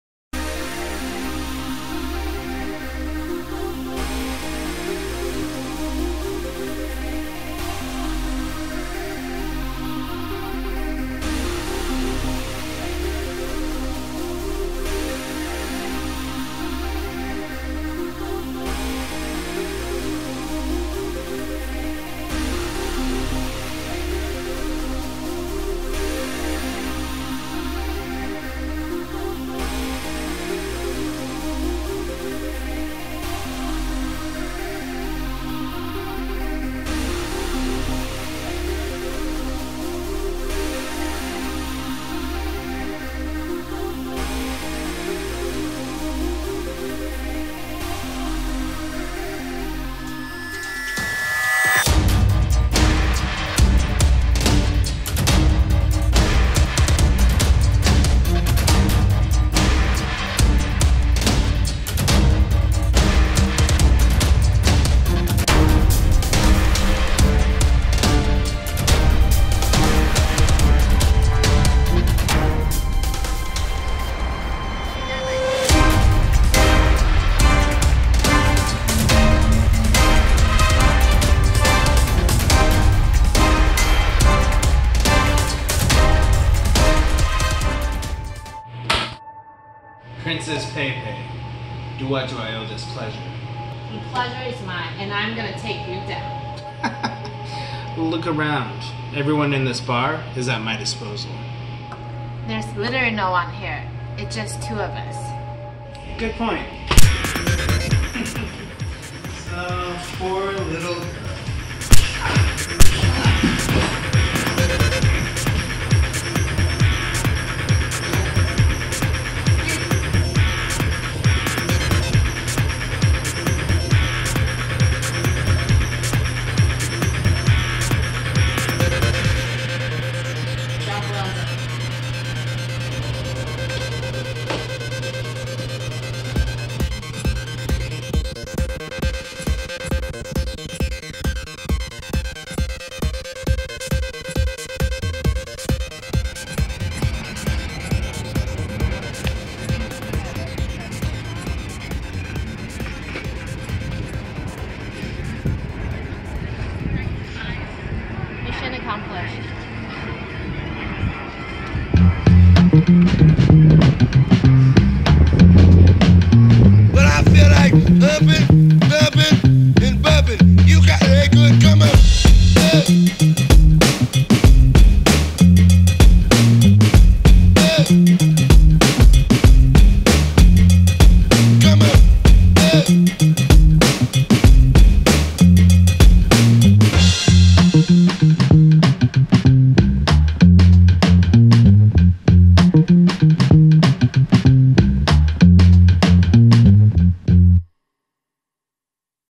Agent Pei - Pei
When Agent Pei-Pei is activated, she seeks out the ruthless mob boss to take him out, before he can do any more harm.

(We were in NYC and wanted to make a film, so this was very quickly thrown together).

ROCK CANYON CREW